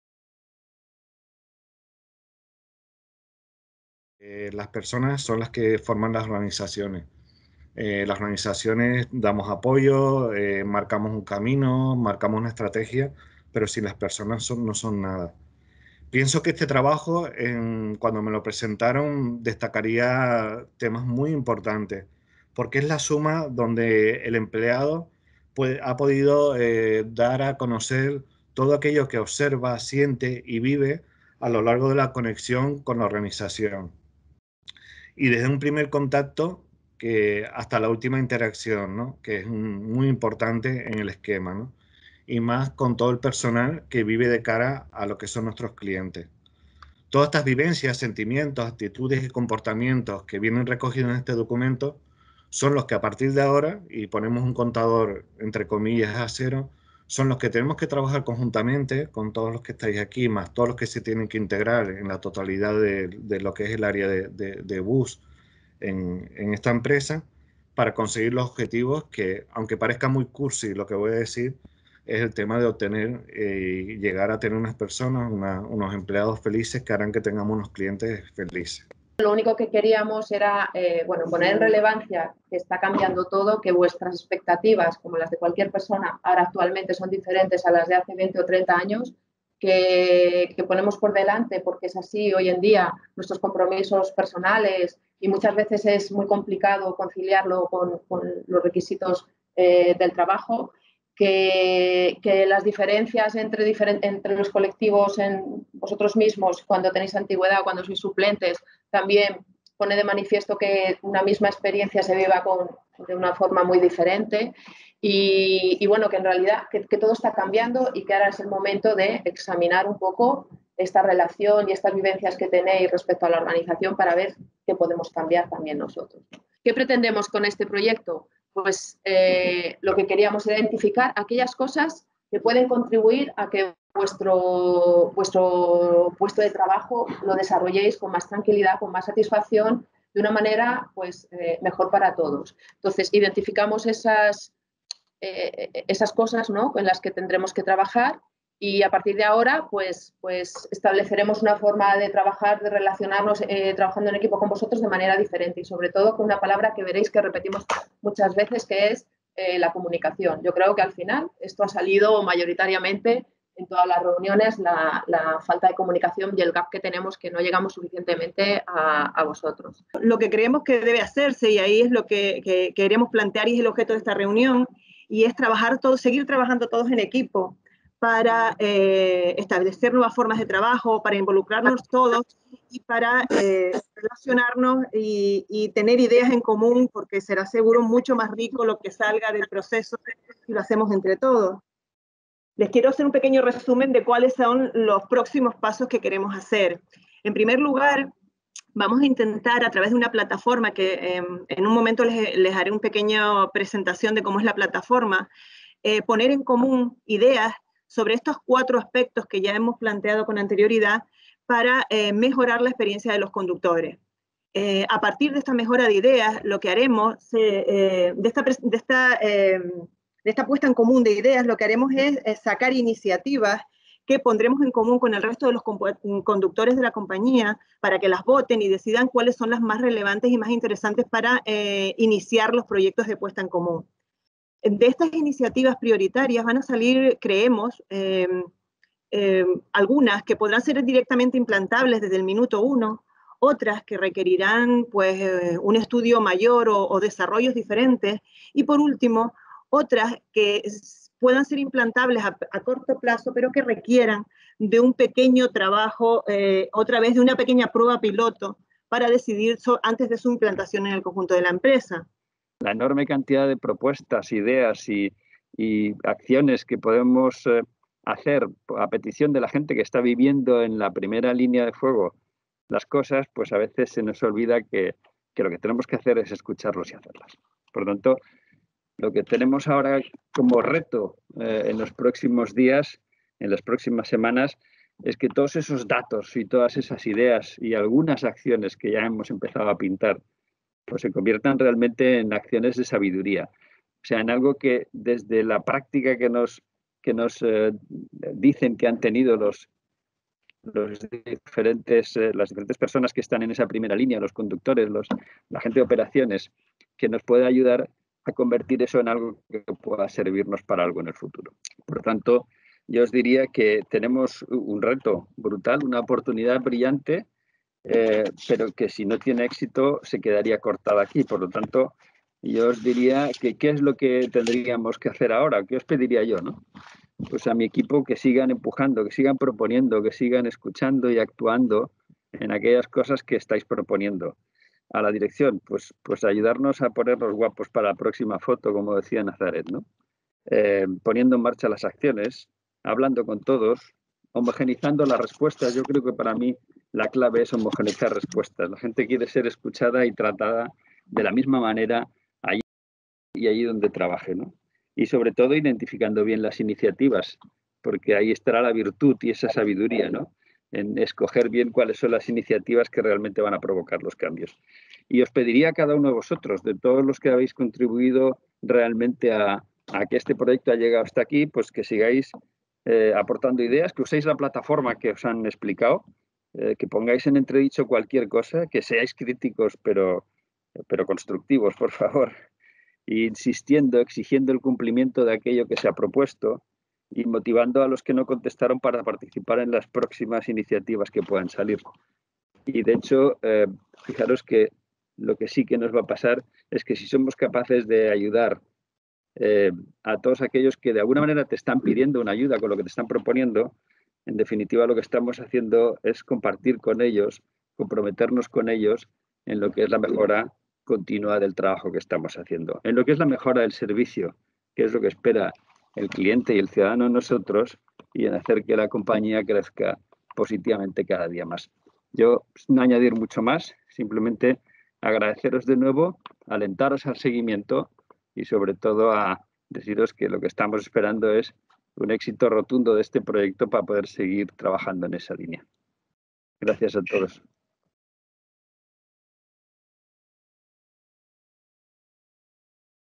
Projecte d'Experiència d'empleat "Conductor/a de Bus"
Invitació a participar aportant idees als reptes identificats en el Mapa de l’Experiència dels Conductors/es Bus.